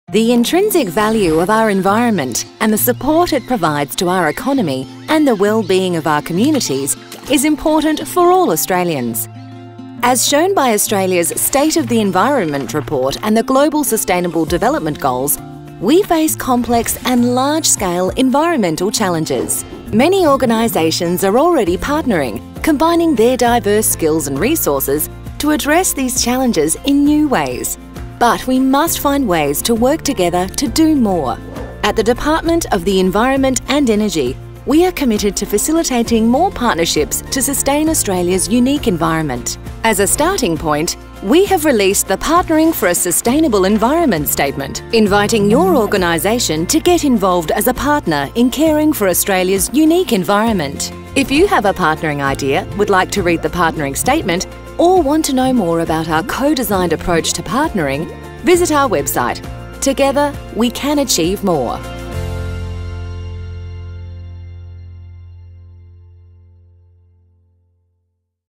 An invitation to partner for a sustainable environment
The Department of Environment and Energy invites your organisation to bring forward your partnering ideas to address Australia’s environmental challenges in ways that also create social, cultural and economic value.  Check out the video and find our more at our